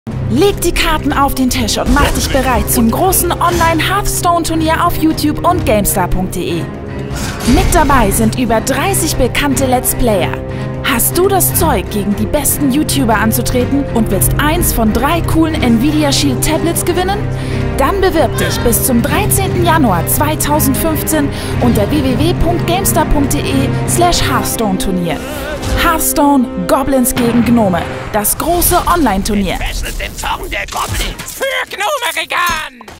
Bewirb Dich beim Gamestar HearthStone Turnier!
Bewirb dich jetzt beim GameStar.de HearthStone Turnier und ergattere evtl. die Chance auf ein NVIDIA Shield Tablet. Und spiele gegen einen von 30 Let's Playern. Mehr Infos folgen bald!

►fisHC0p Post kannst du hier hin schicken:
fisHC0p TV ♣ Postfach 350165 ♣ 10210 Berlin

♣ fisHC0p TV - Let's Play / Gameplay Kanal ♣